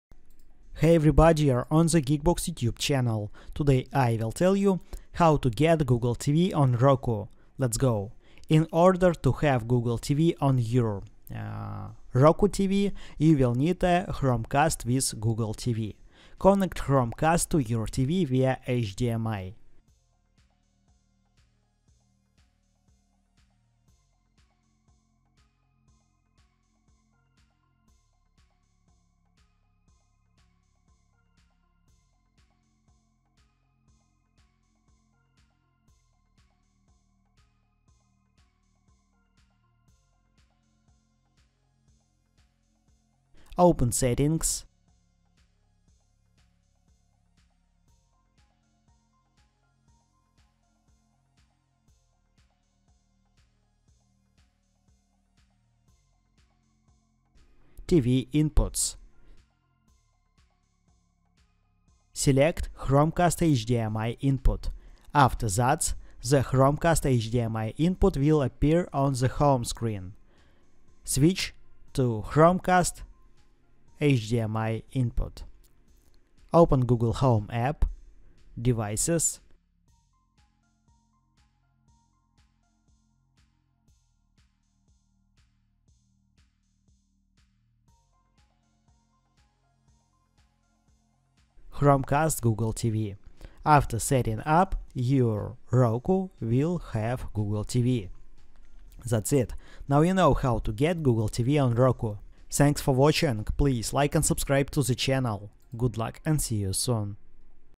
How To Get Google TV On Roku
How To Get Google TV On Roku 2024
Today I will tell you How To Get Google Tv On Roku (hisense,tcl,onn,stick,box)
let's go
In order to have google TV on your TV, you will need a chromecast with google TV
Connect chromecast to your TV via HDMI
open settings
tv inputs
select chromecast hdmi input
After that, the chromecast hdmi input will appear on the home screen
Switch to chromecast hdmi input
open google home app
devices
chromecast/google tv
After setting up, your roku will have google TV
That's it, now you know How To Get Google Tv On Roku